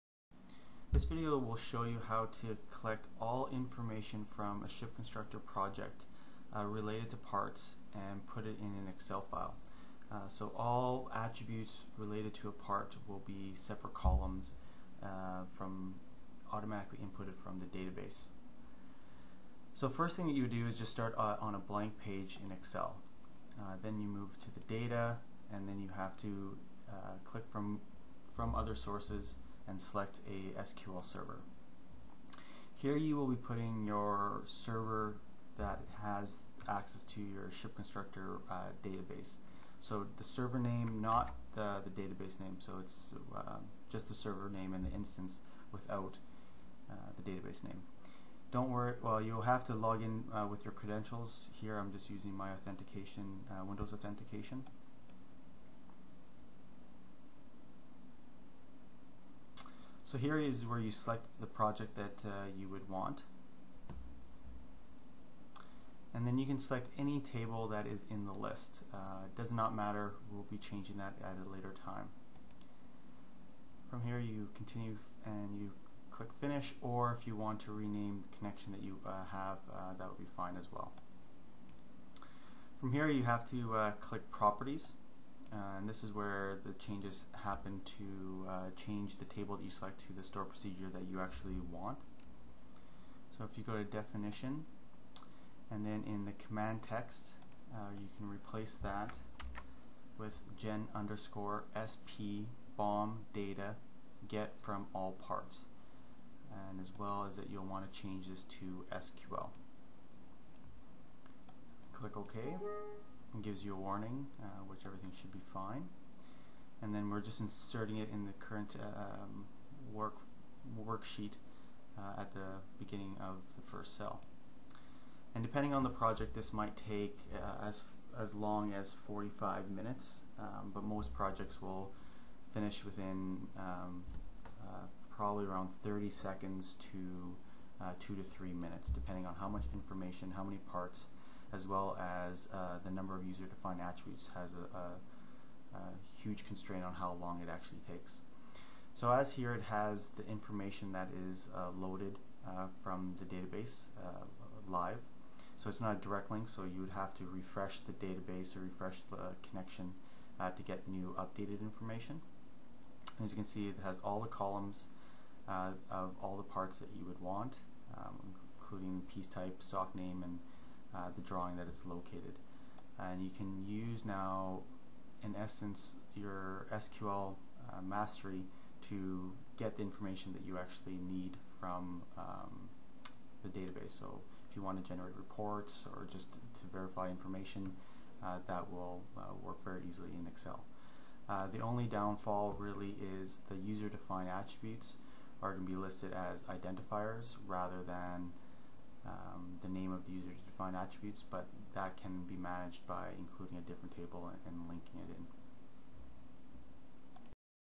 View ShipConstructor Part Information in Excel SC 2013
A video which demonstrates how to get all part information into Excel.